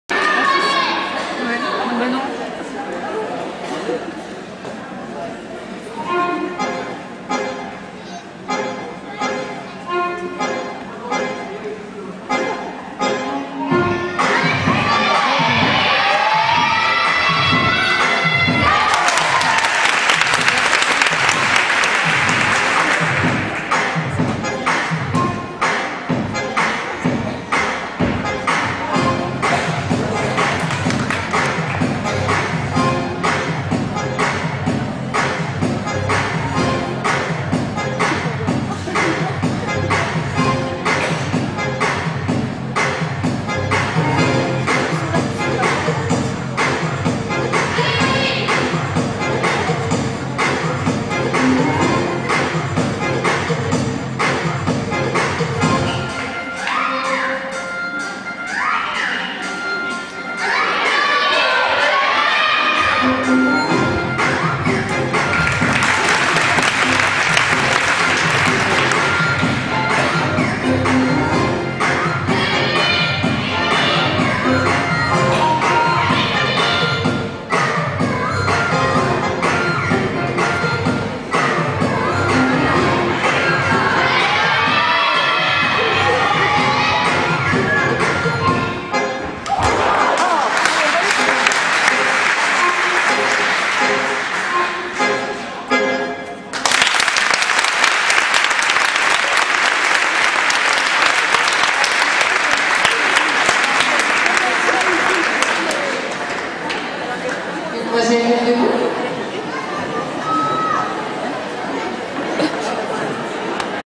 Coline Devillard sol Region équipe 2012
Elle finit 6ème avec l'équipe de Digoin à Blanzy